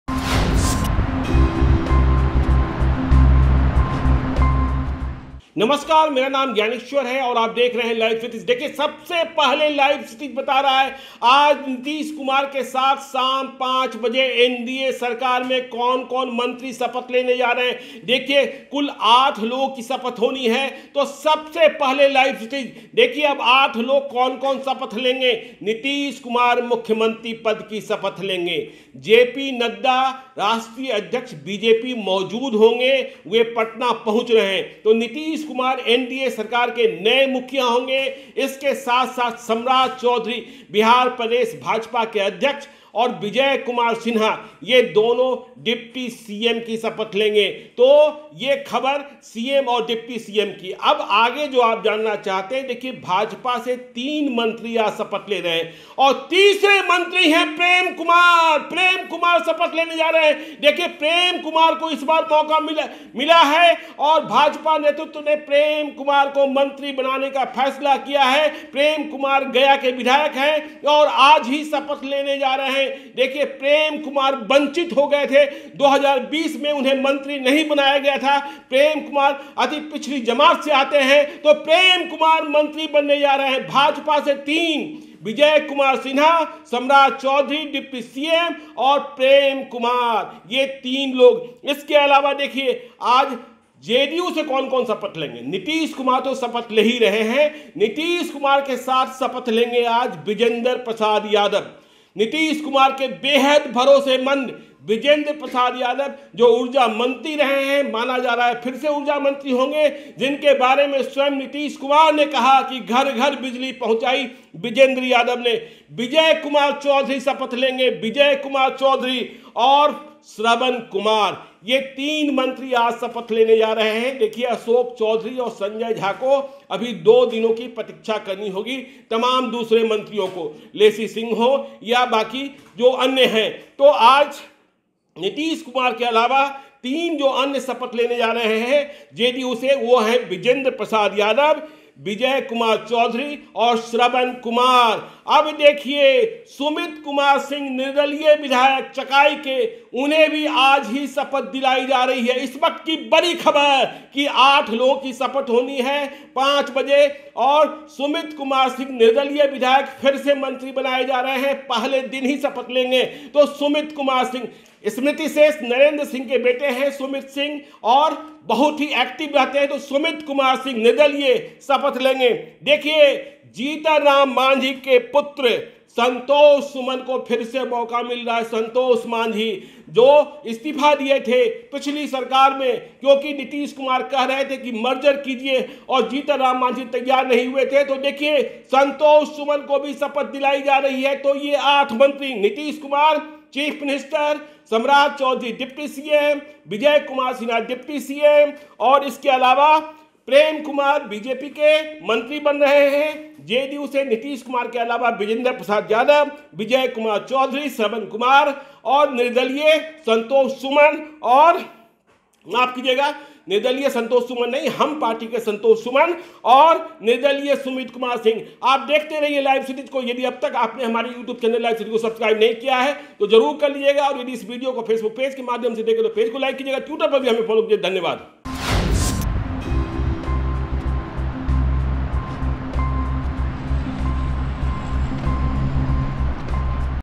Nitish Kumar के नए मंत्रियों की पूरी लिस्ट, बता रहे Gyaneshwar, इन्हें मिला मौका, चौंकाने वाले नाम
Nitish Kumar के नए मंत्रियों की पूरी लिस्ट, बता रहे Gyaneshwar, इन्हें मिला मौका, चौंकाने वाले नाम | Bihar News | LiveCities